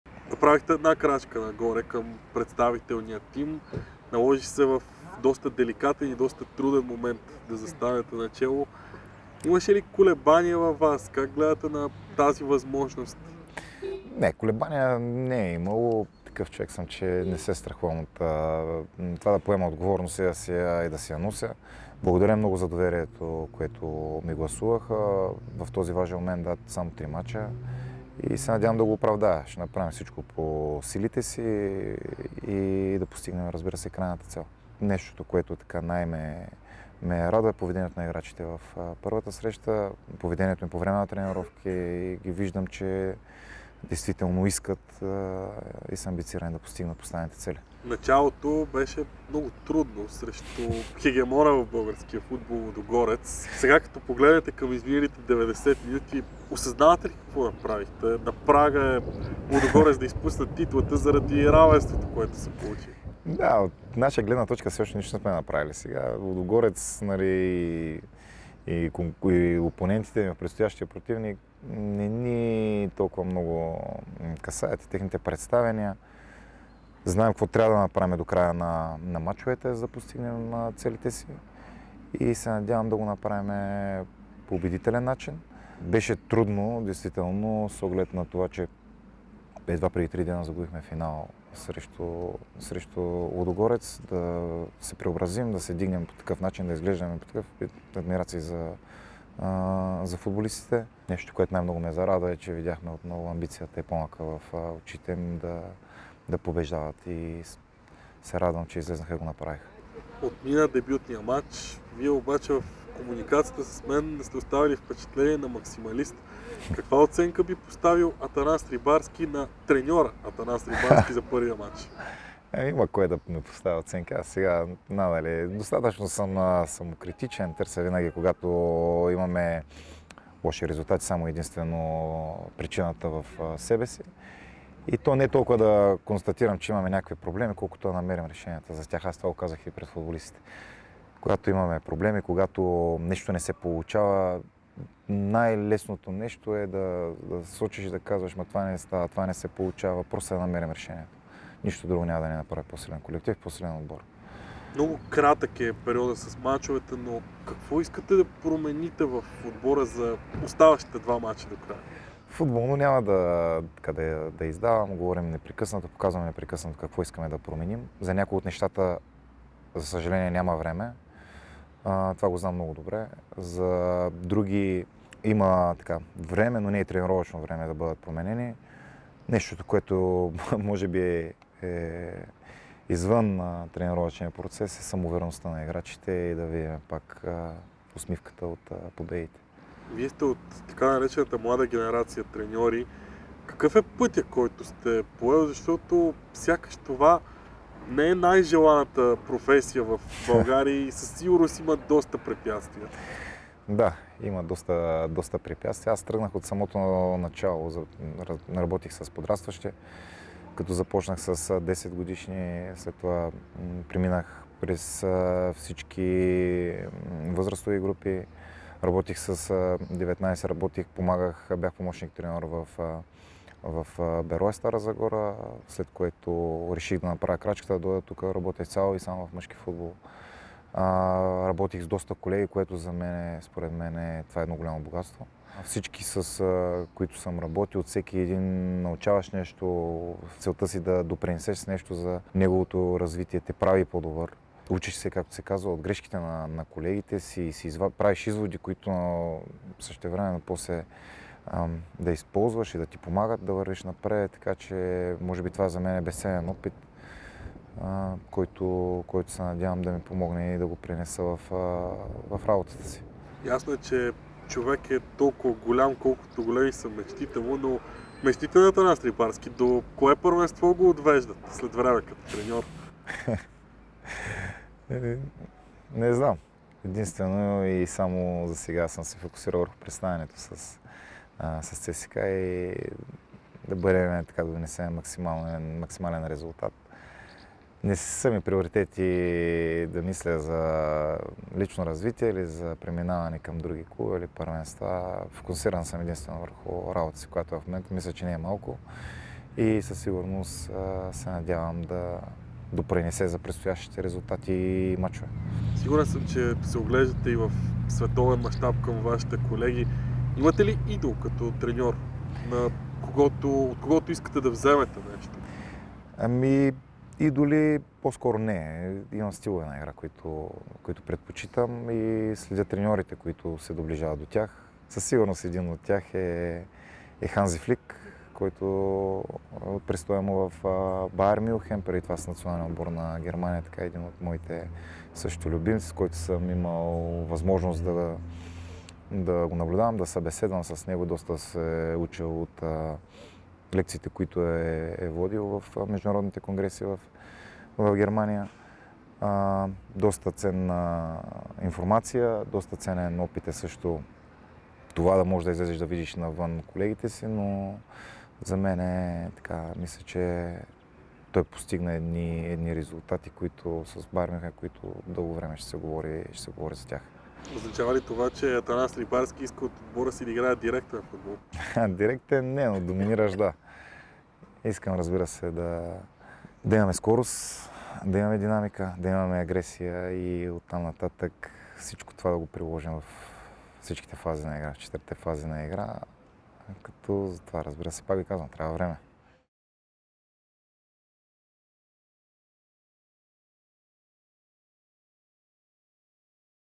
🗣️ Атанас Рибарски говори за мечтите си в треньорската професия
🔴 Атанас Рибарски стартира с равенство престоя си като старши треньор на ЦСКА. Резултат, който може да наруши статуквото в българския футбол.

Каква е равносметката му от изминалия дебют, какви са плановете и мечтите му за бъдещето - вижте във видеото.

👉 Абонирайте се в YouTube:  @cska1948fc